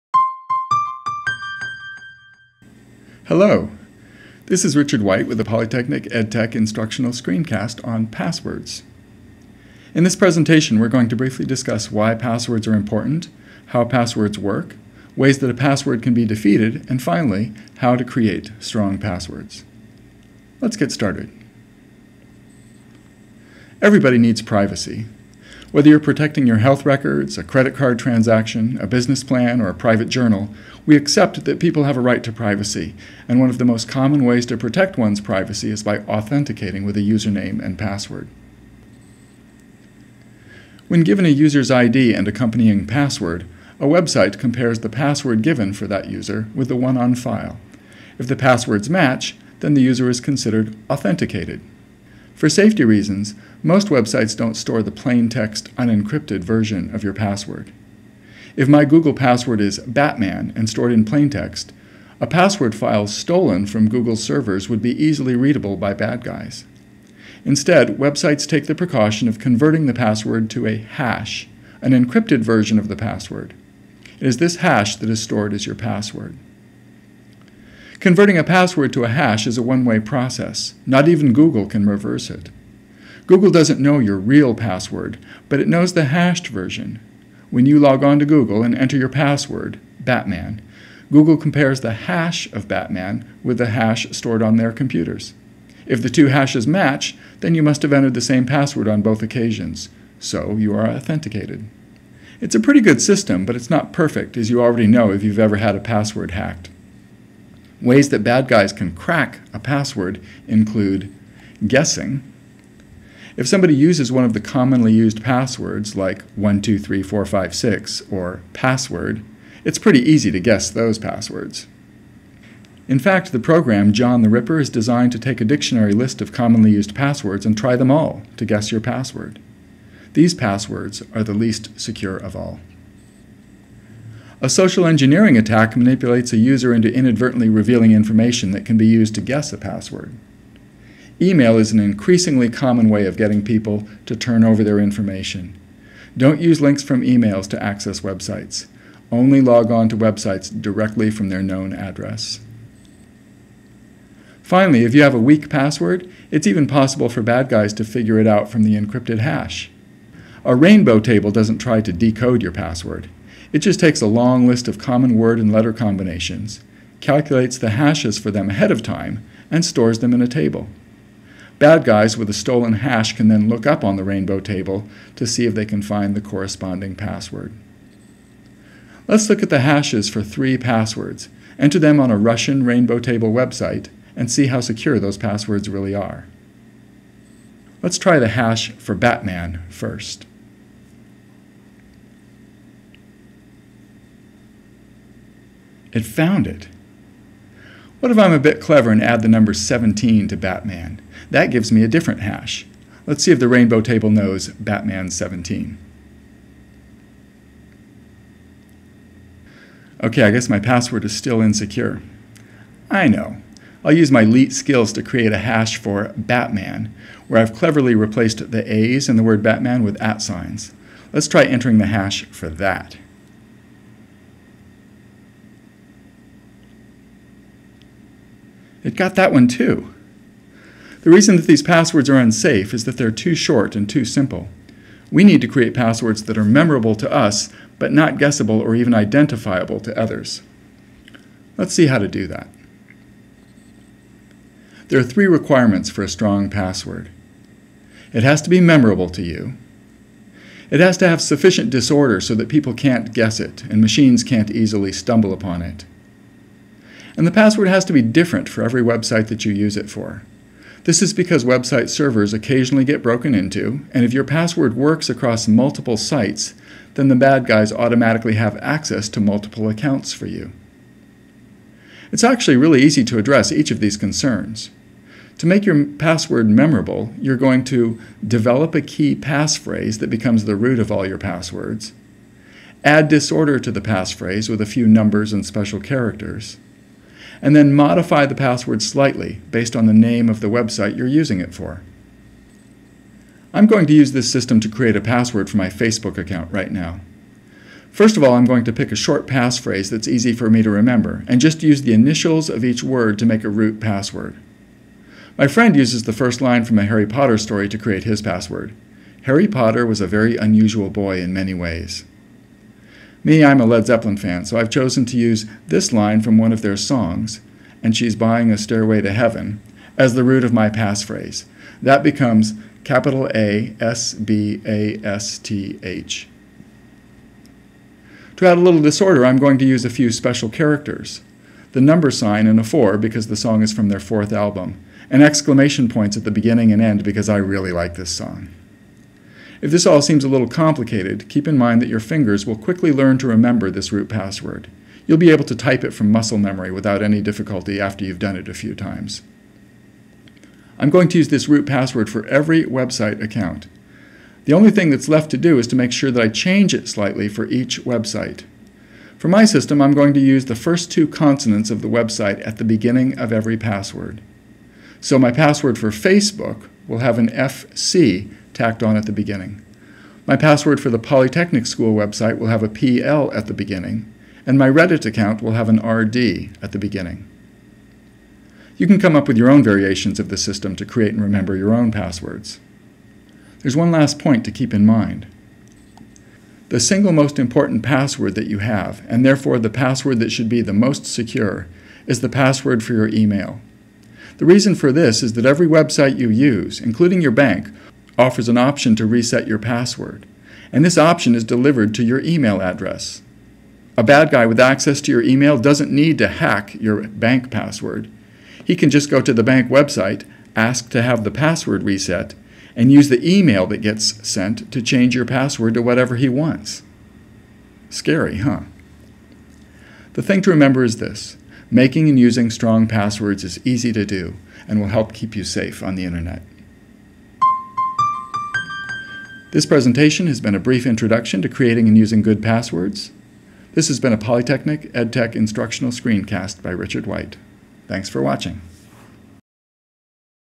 Polytechnic School EdTech - Passwords
An introduction to passwords, for Polytechnic's Educational Technology Instructional Screencasts